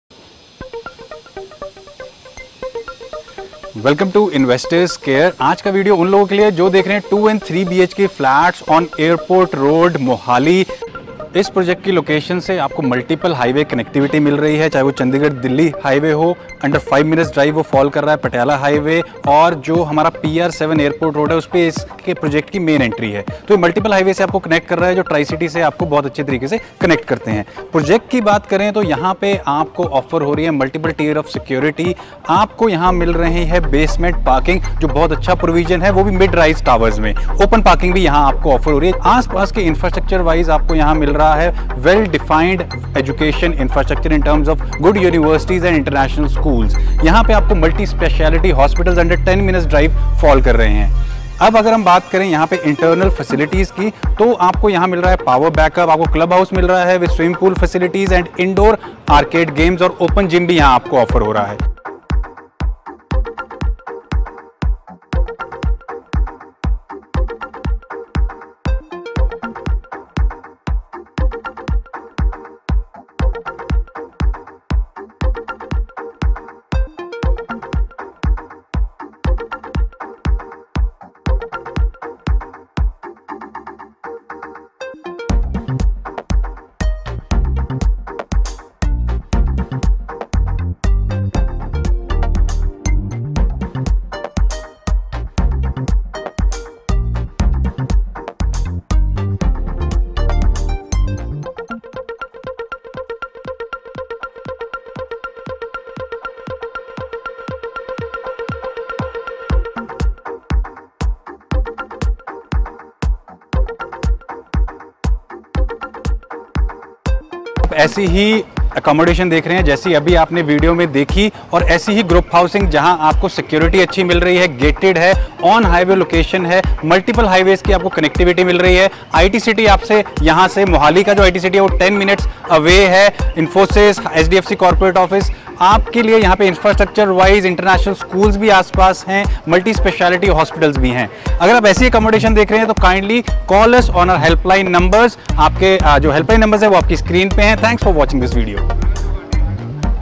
2BHK Apartment on Airport Road Mohali Just 2 mins from International Airport Nearing Possession
Ready to move in Units available